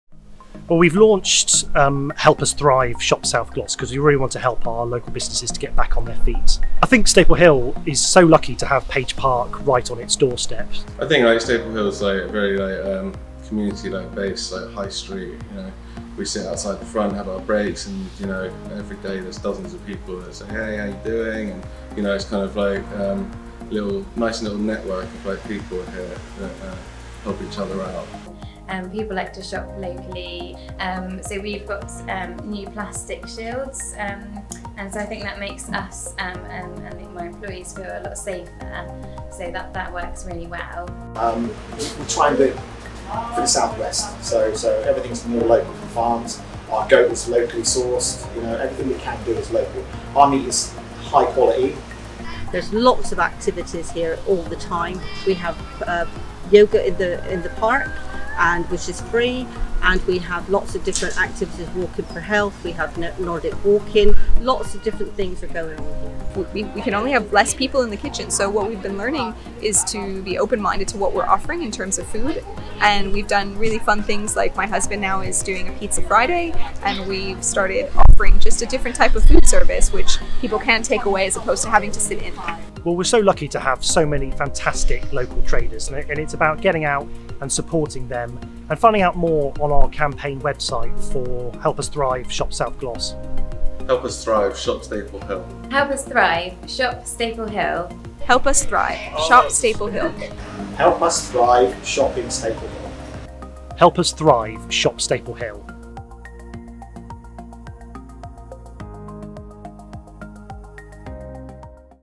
Help us thrive: Shop Staple Hill
A short video about Staple Hill High Street, which is perfect for shopping, and sitting outside enjoying a drink at one of its many vibrant cafes and pubs.